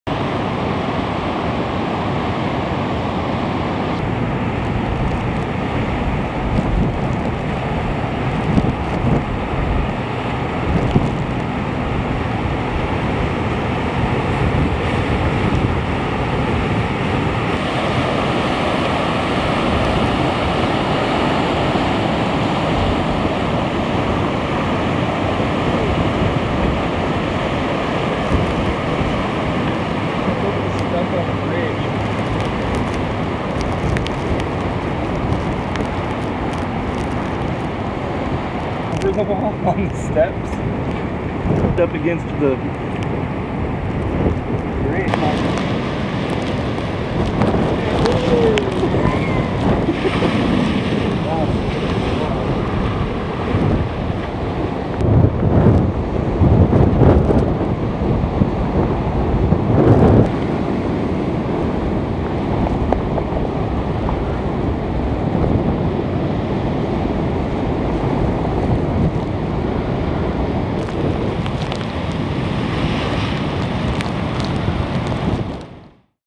King Tide hits Kyllo's
King Tide struck Lincoln City Tuesday, sending logs crashing underneath Kyllo's Seafood Grill which sits on the D River.